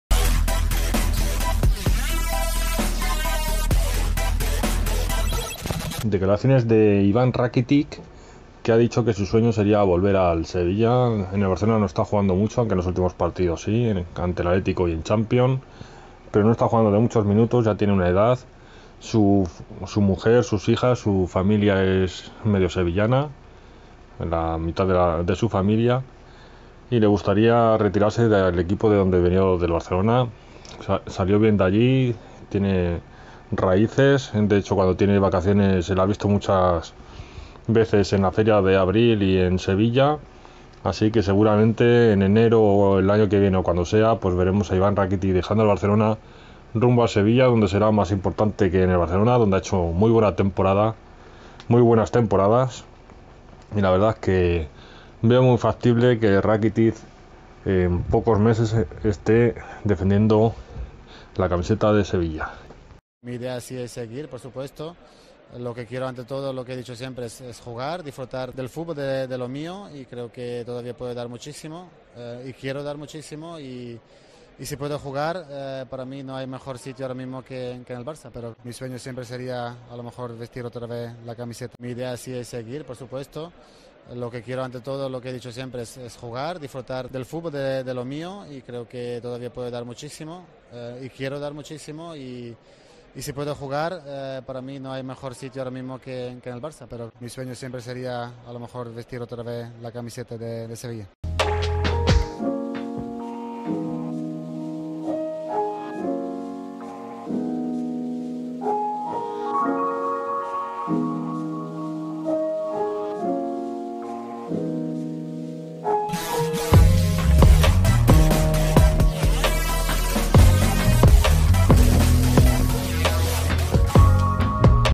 RAKITIC dice que su SUEÑO es volver al SEVILLA
Rakitic: "Mi sueño siempre sería vestir otra vez la camiseta del Sevilla".
El centrocampista del Barcelona esta jugando poco esta temporada